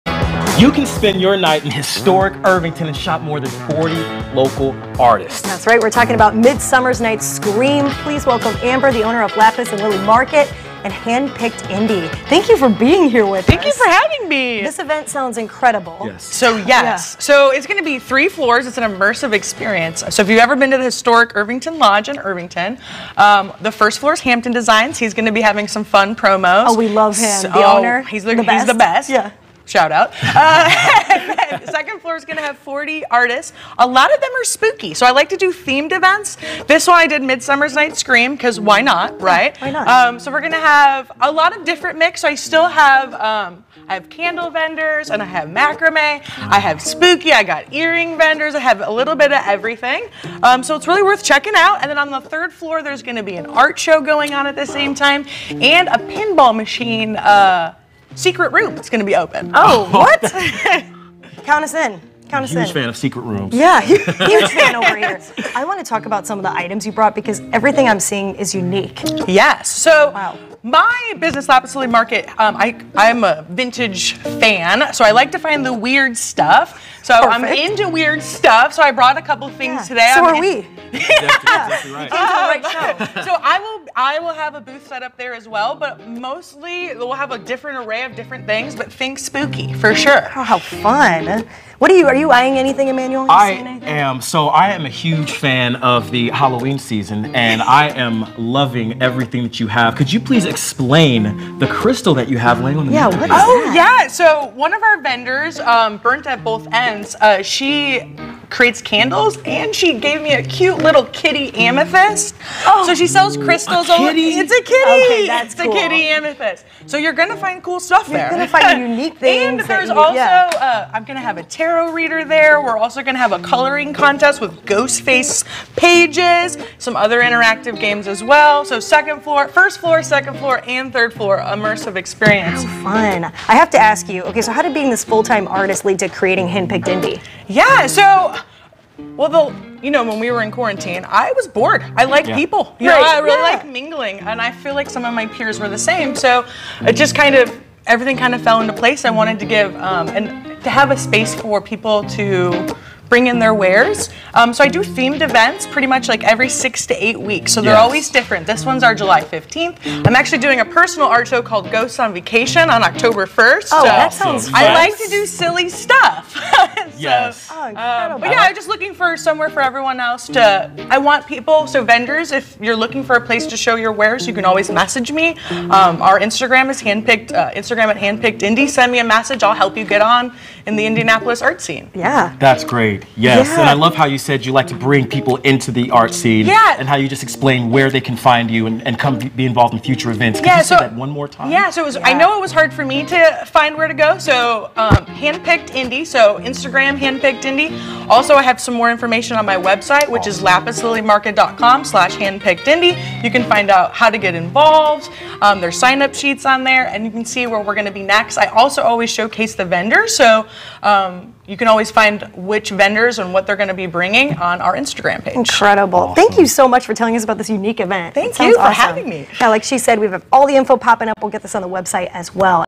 Indy Now
Amber Gyselinck the owner of Lapis Lily Market and Handpicked Indy stopped by the studio to share more about the unique vintage and handmade market coming up next weekend.

The event, Midsummer Night's Scream is taking place on Friday, July 15th from 6 p.m. until 9 p.m. at 5515 Washington Street.

To learn more visit lapislilymarket.com.